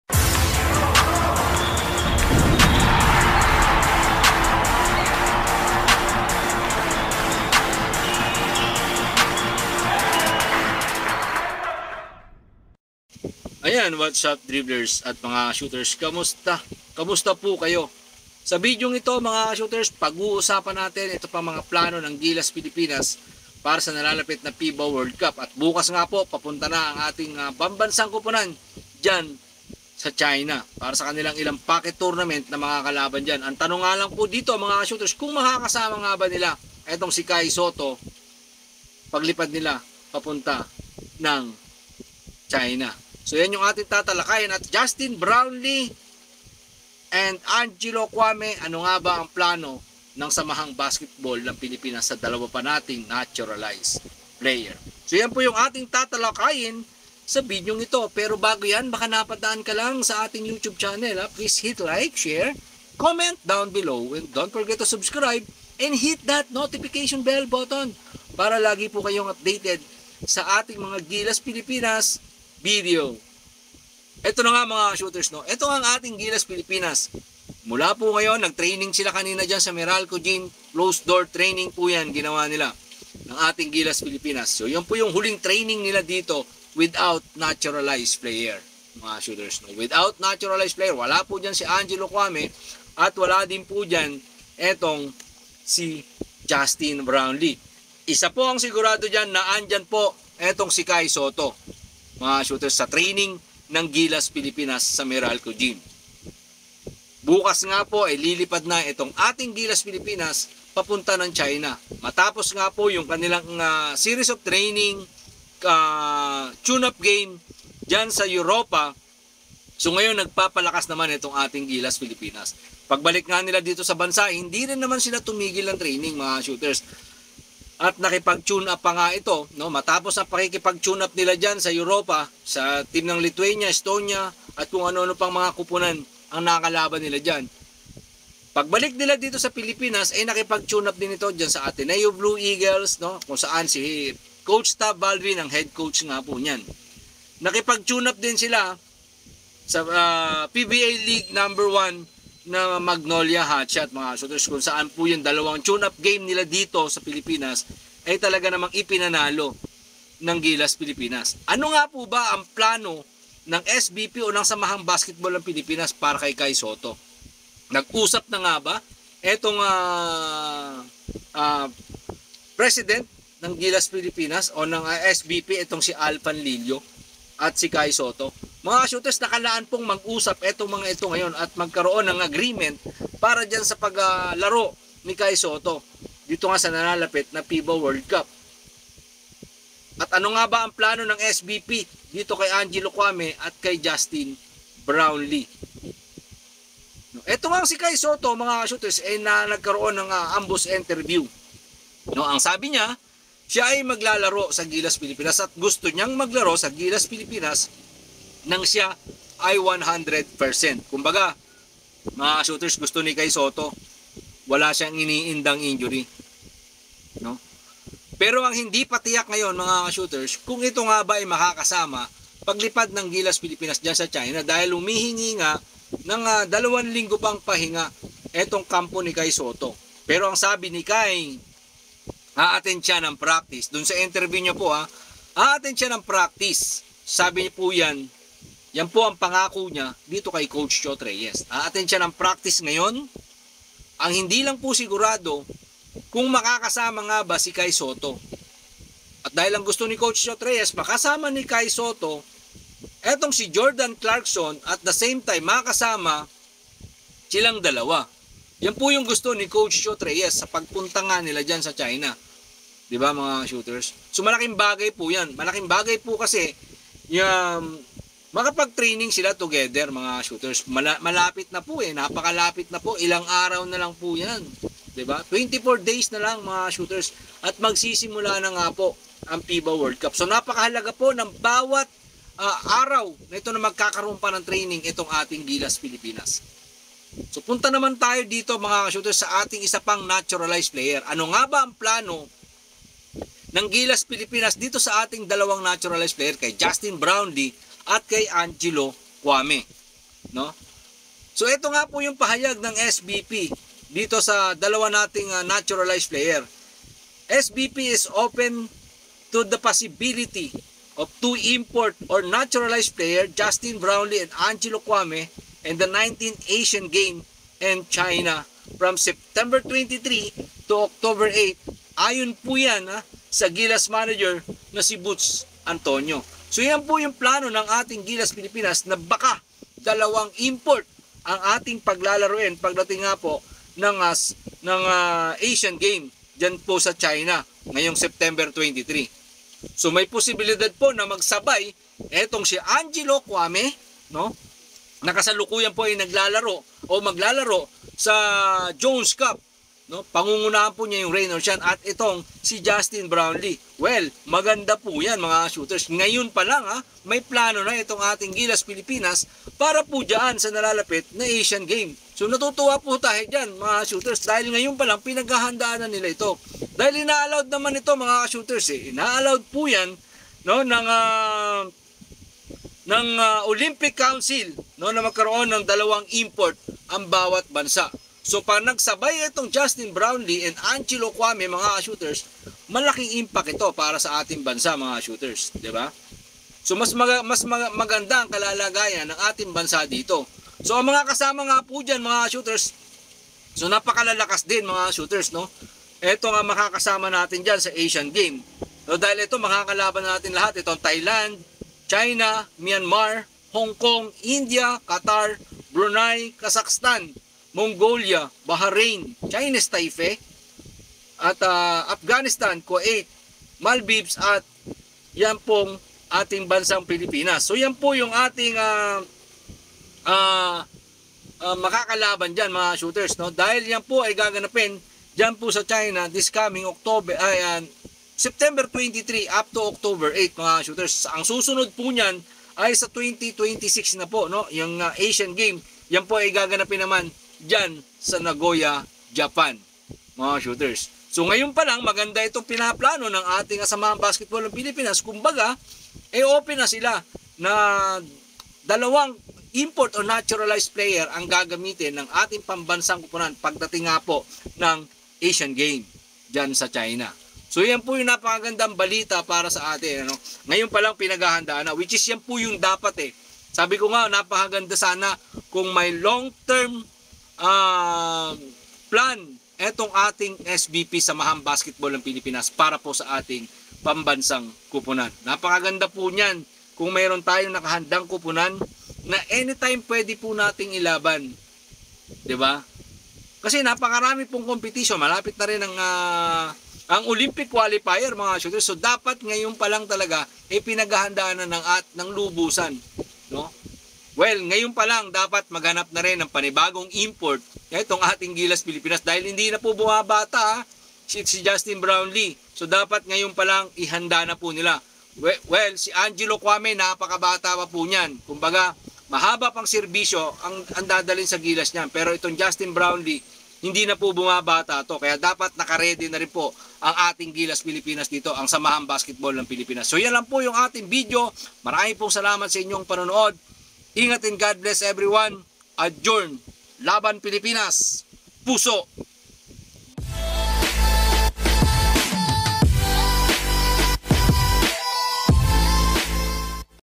GILAS PILIPINAS UPDATE FOR KAI SOTTO / TWO NATURALIZED PLAYER'S SA ASIAN GAME SA SEPTEMBER JB & Ak
Sabi NGA Ng SBP samahang basketball Ng PILIPINAS maaring DALAWA Ang naturalized player na gagamiti sa Asian game updates for Kai sotto